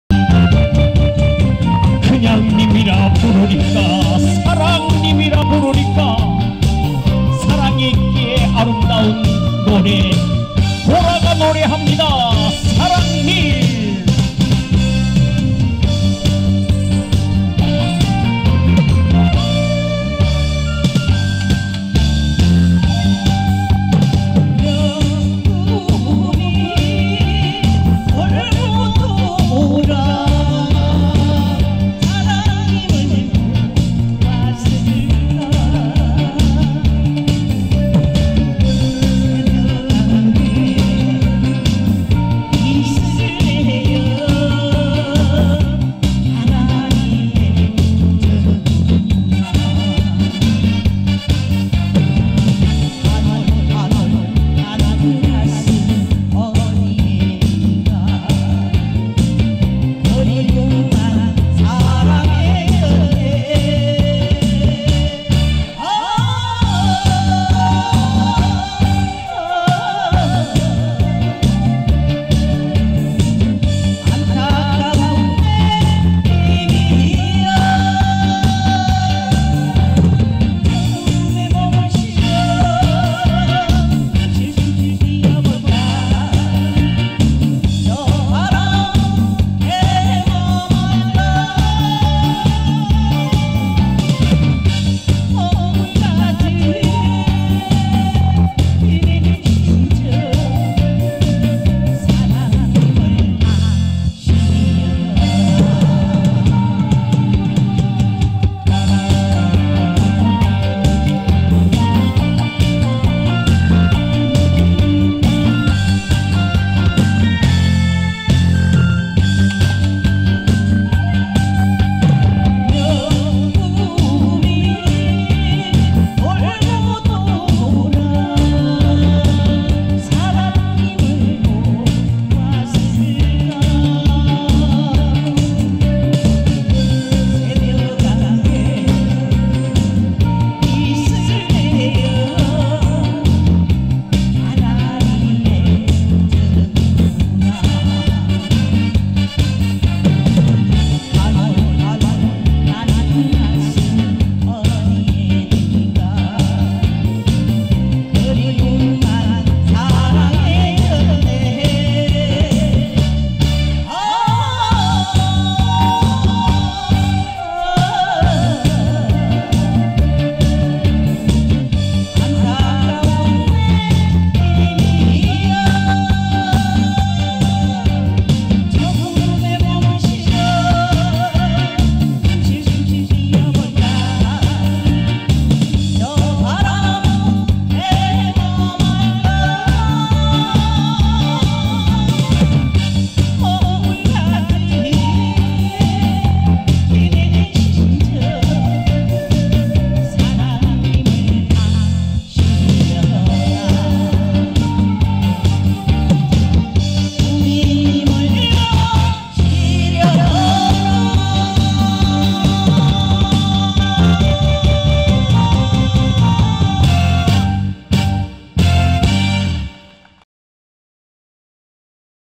가수 보라♬사랑님☞힐링 콘서트▶2021년5월9일
♪힐링콘서트
주최:한울림예술단
장소:단장면 고례댐 생태공원